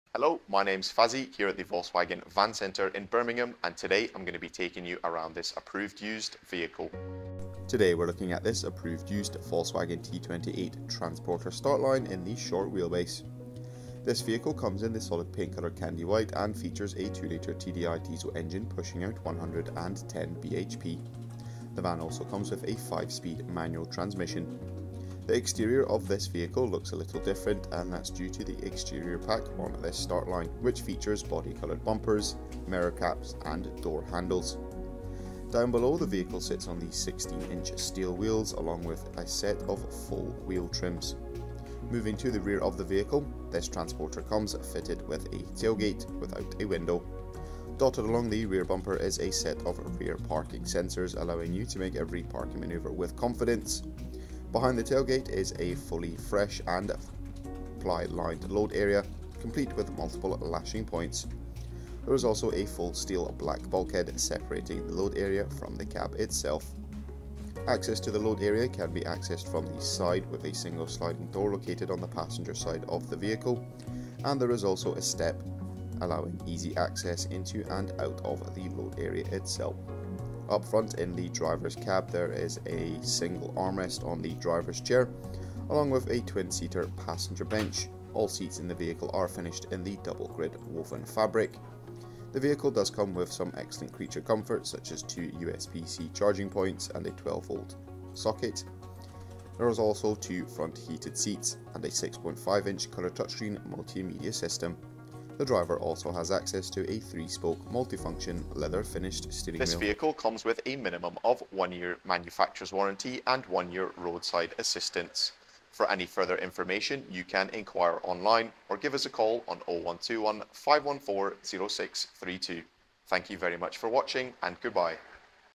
Approved Used Volkswagen Transporter T28 SWB Diesel 2.0TDI 110 Startline Business Pack Exterior Pack
Reg: BF23JMV
Colour: 'Candy White' in Solid Paint
Fuel: Diesel
Transmission: 5 Speed Manual
Engine Size: 2.0L
Mileage: 14 Miles

This van comes with:
Exterior Features
Candy White-Solid Paint
Body coloured bumpers
Body coloured mirror caps
Body colored door handles
Tailgate-solid-without bulkhead
Single sliding side load door on passenger side
Parking sensors-rear
Safety
Forward collision warning incl. autonomous emergency braking
Doors
Tailgate without window
Driver Convenience
Leather-wrapped multi-function steering wheel (3-spoke)
Cruise control with speed limiter
Interior Features
Electronically controlled air conditioning system
Heated front seats
Rubber floor covering in cab
Tailgate/trunk lid release from inside (electrically)
Audio/Communication
6.5" colour touch screen multimedia
DAB digital radio
App Connect
Apple Car Play
Android Auto
Twin USB 'C' interfaces
Bluetooth connectivity
Exterior Pack
Business Pack
Trim
Armrests for divers seat
Heated driver and front passenger seats
Double front passenger seat
Security
Thatcham category 1 cab alarm system
2 remote control folding keys
Paint - Solid
Solid paint-Candy White
Trim - Cloth
Double grid woven fabric - Titanium/Palladium black
Wheels
17" Devonport alloy wheel option available-Limited Availability-Please Ask For Details
Steel spare wheel with original equipment tyre